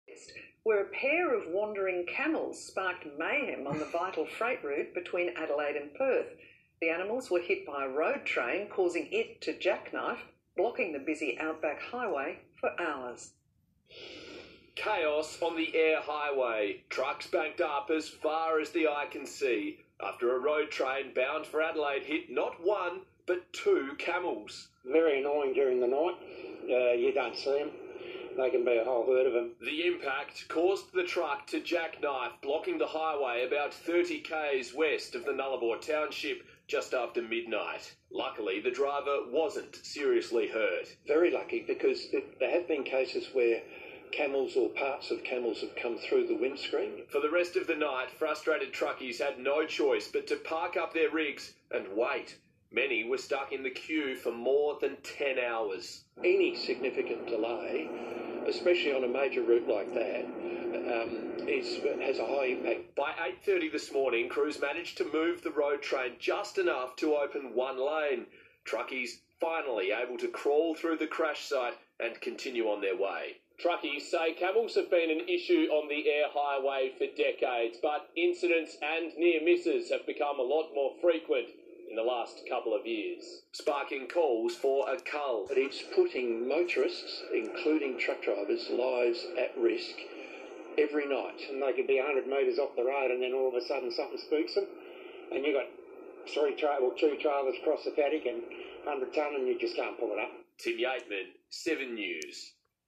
Supply back up due to camels.
If this was real, there would be photos of the truck and the truck would be smashed. The real message is a deliberate back up of supplies by 10 hours in a just in time delivery system. The police and media have a habit of telling porkies.